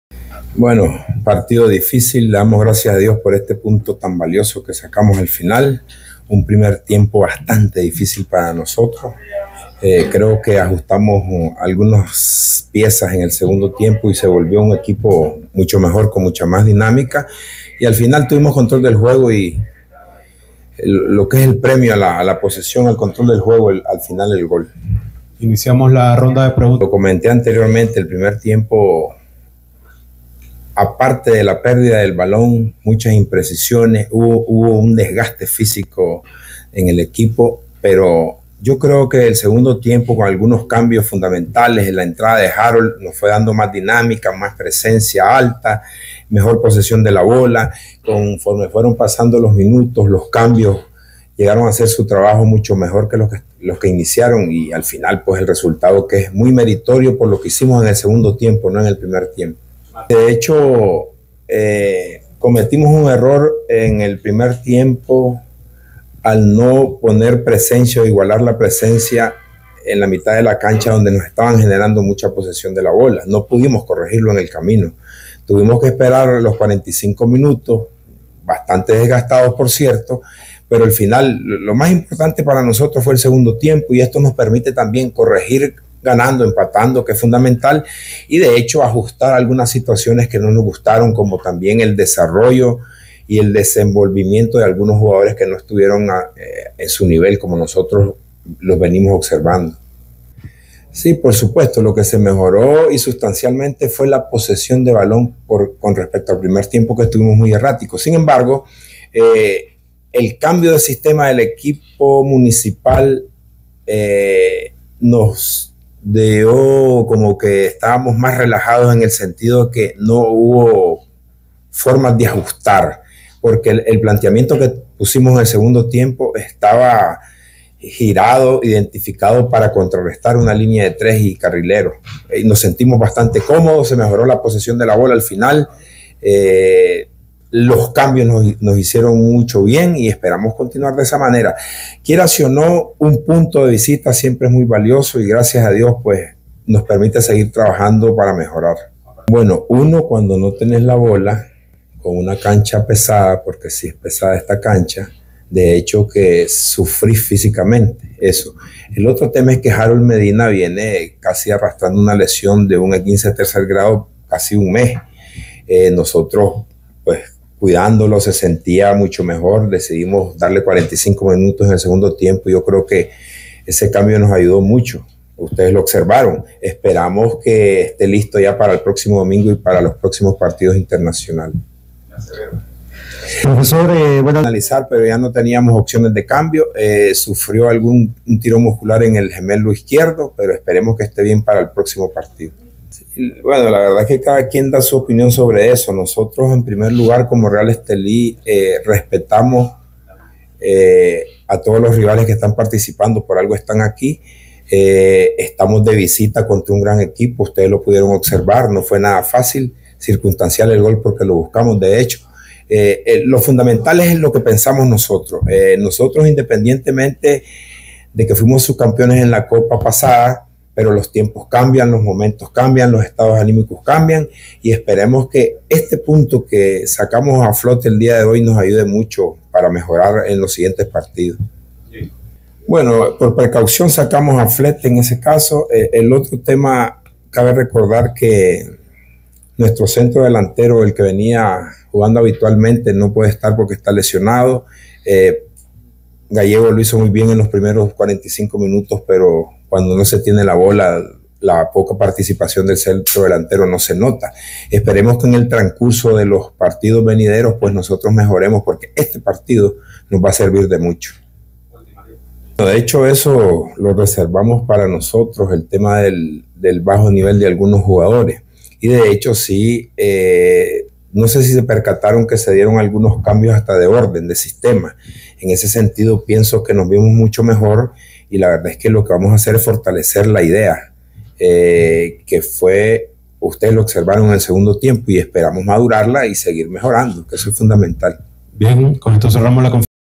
Otoniel Olivas en conferencia de prensa pospartido entre Real Estelí y CSD Municipal.
Otoniel Olivas explicando las razones de el mal desempeño de Real Estelí en el primer tiempo.